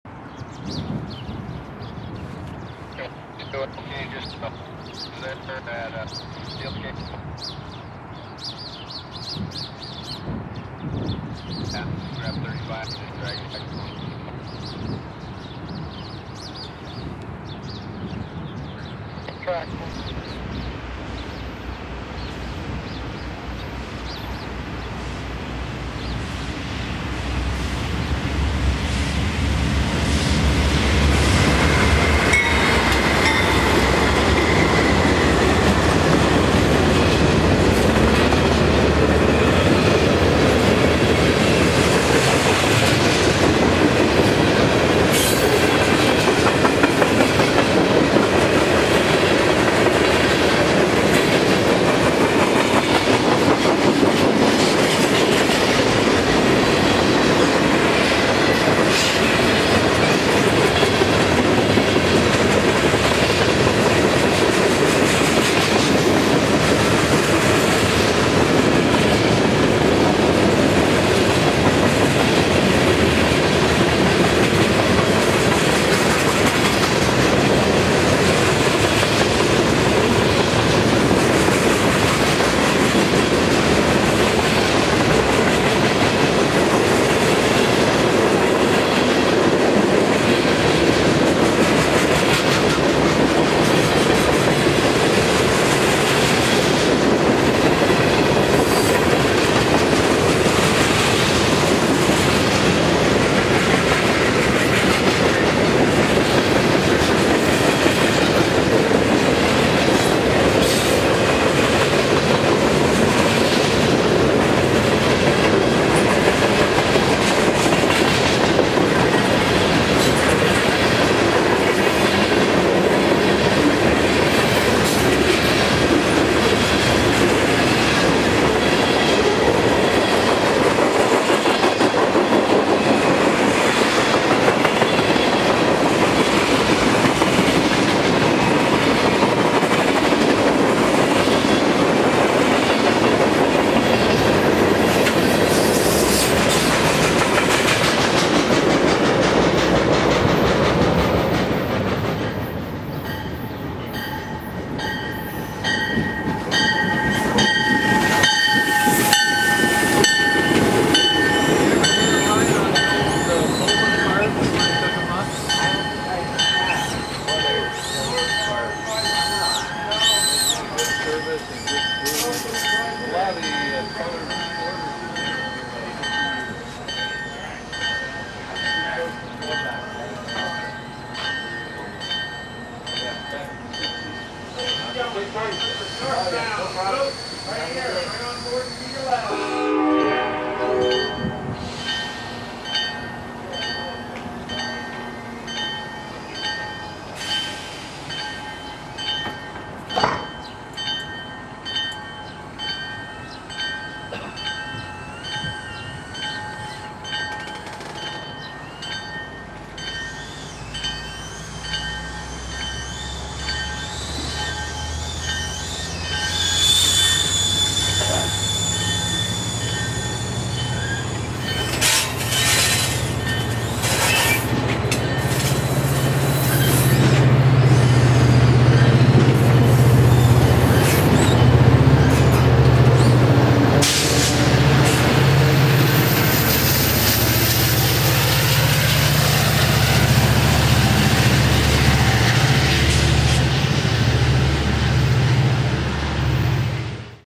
BNSF #5340 & Amtrak #500
2 trains meet at Kelso.